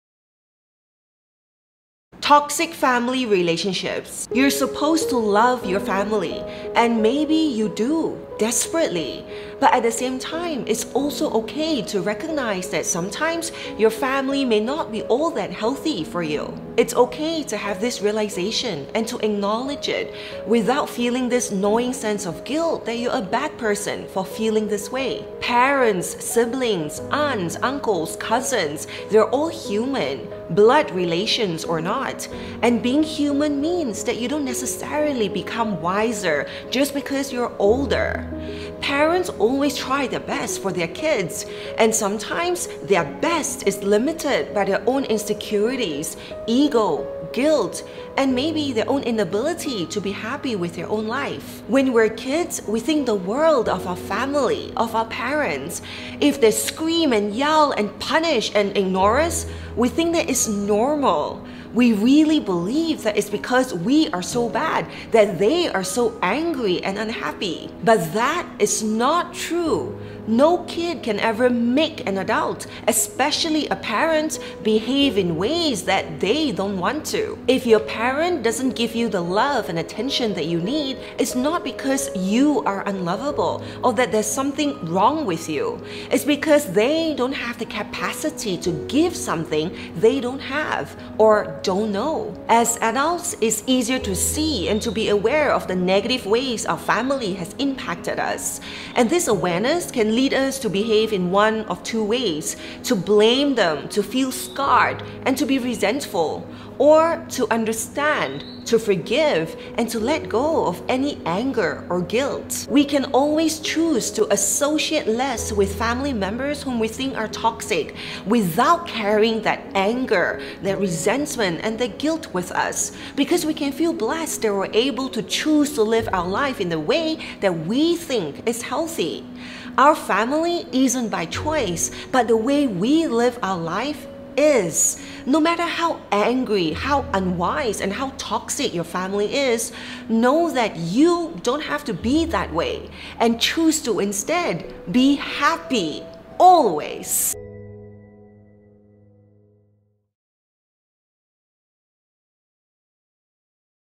Toxic Family Relationships - Happiness Vlog
You're not a bad person if you recognise that your family can be toxic. Watch this XO

Learn how to create positive change and be the best version of yourself with my 30 day happiness program!:

To Read:
Toxic family relationships.

You’re supposed to love your family, and maybe you do, desperately. But sometimes, it’s okay to recognise that your family may not be all that healthy for you.

It’s okay to have this realisation and to acknowledge it, without feeling this gnawing guilt that you’re a bad person for feeling this way.

Parents, siblings, aunts, uncles, and cousins - they’re all human, blood relations or not. And being human means that you don’t necessarily become wiser just because you’re older.

Parents always try their best with their children, it’s just that for some parents, their best is limited by their own insecurities, ego, guilt and the inability to be happy with life.

When we’re kids, we think the world of our parents and family members. If they’re upset and scream and ignore and punish, we think it’s normal. We can really believe that it’s because we’re so bad that they’re so angry and unhappy.

But that is not true. No kid can ever ‘make’ an adult, especially a parent, behave in ways that they don’t want to.

If your parent doesn’t give you the love or attention you need, it’s not because you are unlovable or that there’s something wrong with you, it’s because they don’t have the capacity to give something they don’t know or don’t have.

As adults, it’s easier to see and to be aware of the negative ways our family has impacted us.  This awareness can lead us to react in one of  two ways - to blame them and feel scarred and resentful, or to understand, forgive and let go of anger or guilt.

We can always choose to associate less with toxic family members without carrying this anger or resentment or guilt, because we feel blessed that we are able to choose to life in the way that we think is healthy.

Our family isn’t by choice, but the way we live our life is. No matter how angry, unwise or toxic your family is, know that you don’t have to be that way, and choose to instead, be happy, always.